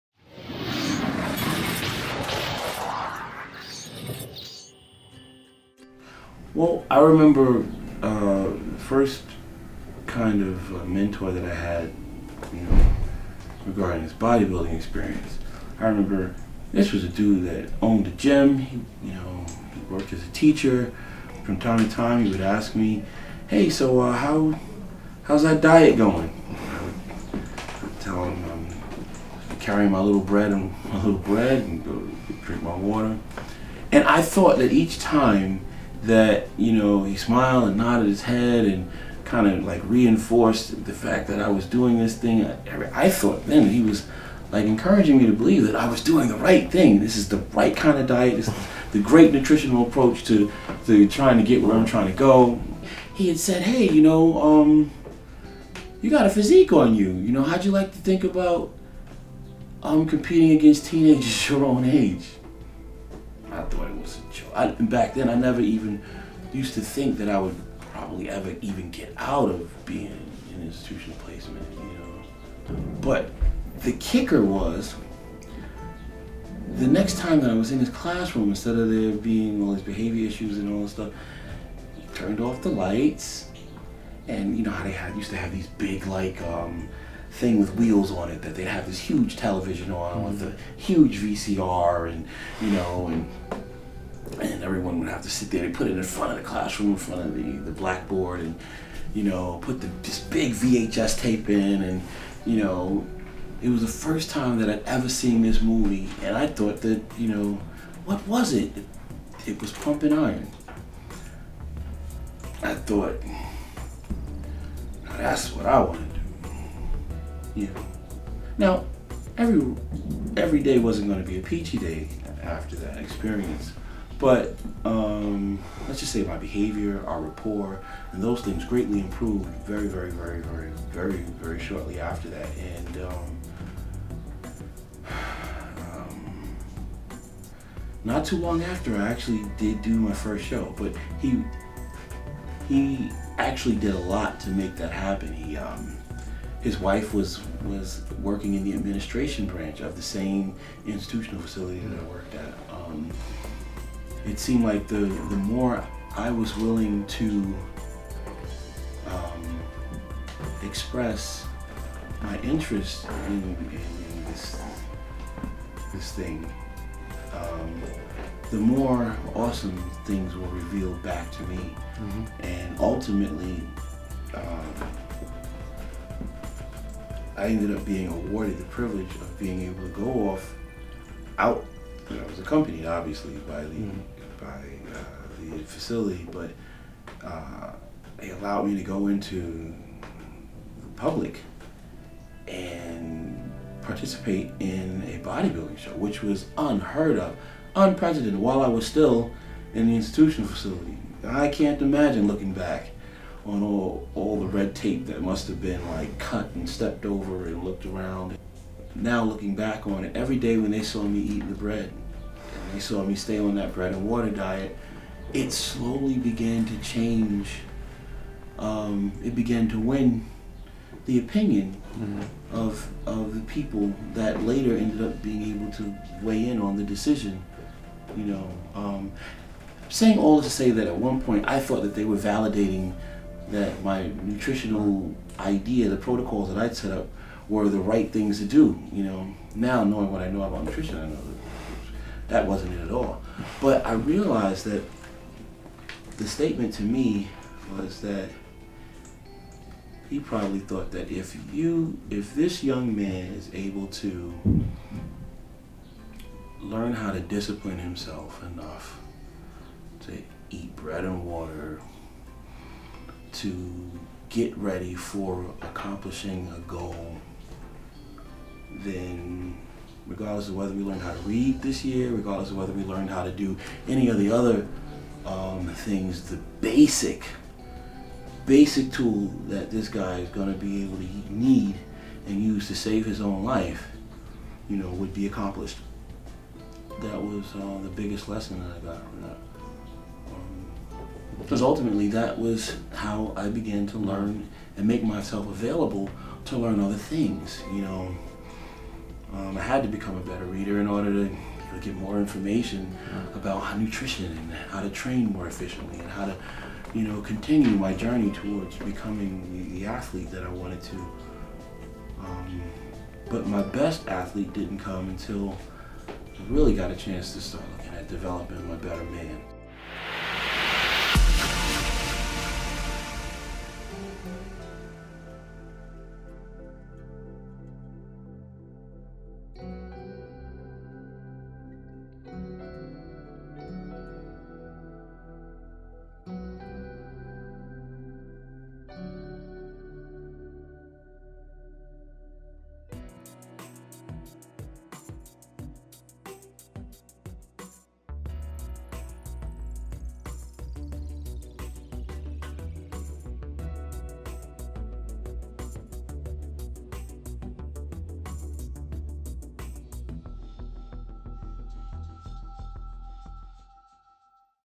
How Kai Greene Found Bodybuilding | Generation Iron
Kai Greene has had a rough upbringing to say the least. The bodybuilder is pretty open about his past and the difficulties he’s been faced with through his young life. In this exclusive behind the scenes interview, Kai Greene talks a bit more on his past, just how he got into bodybuilding and how his life has been forever changed by the sport.